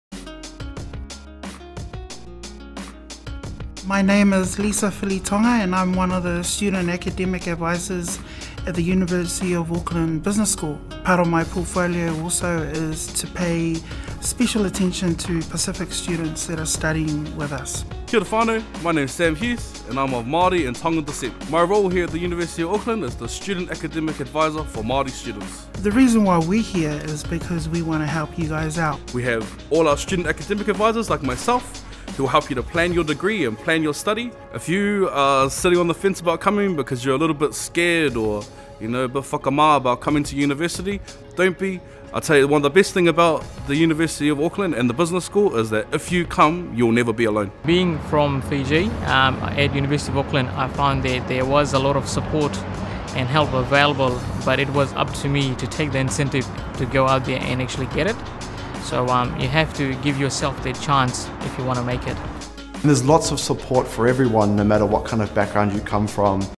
Future Māori and Pacific students - Help and support at the Business School
The Business School has dedicated Māori and Pacific Student Academic Advisors who can provide help and support with courses, grants and scholarships.